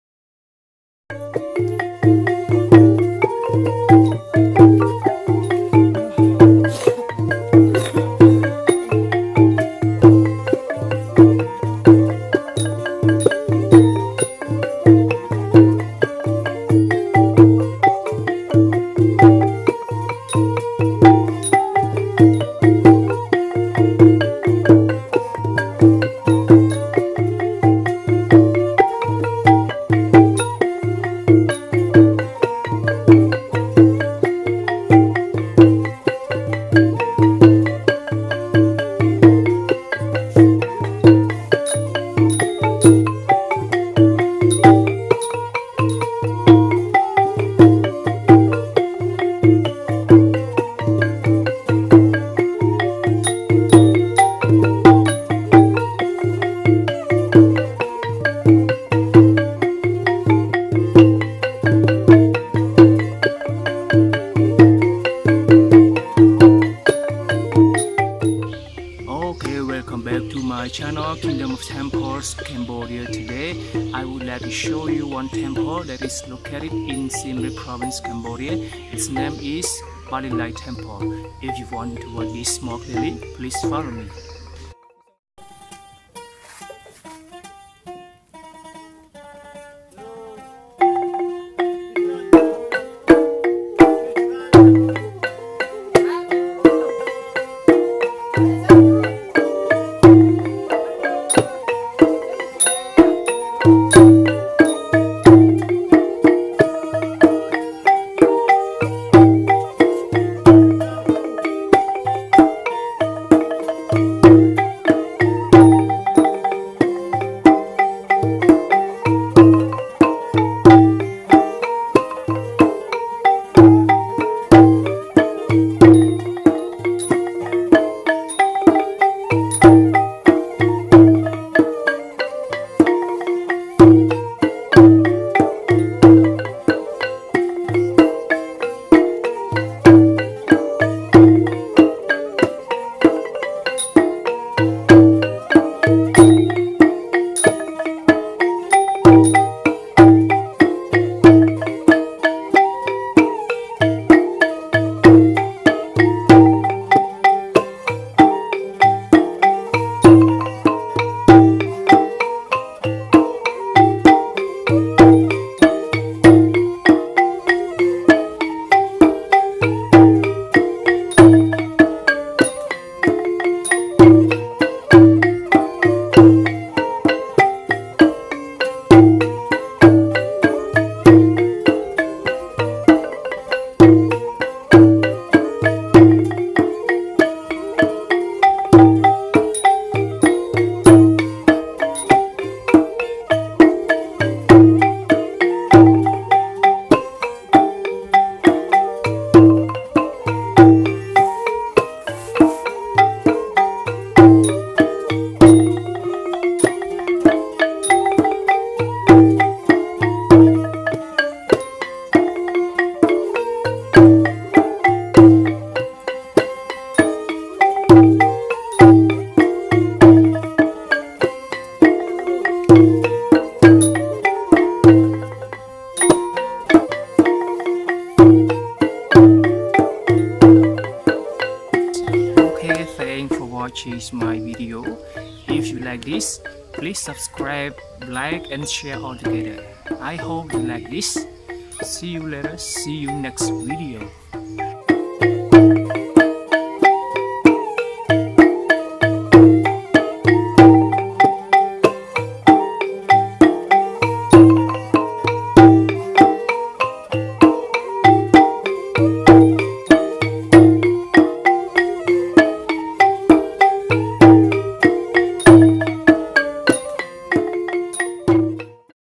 Palilay Temple | Kingdom of Wonder | Siem Reap | Welcome to Cambodia
Welcome to Cambodia
Palilay is a temple at Angkor, Cambodia. It is located in Angkor Thom. It was built at the early of 12th century.
I'm going to take all of you to visit my country, the Kingdom of Wonder, Cambodia that fulls of many temples like Angkor Wat, Bayon, Banteay Srey Temples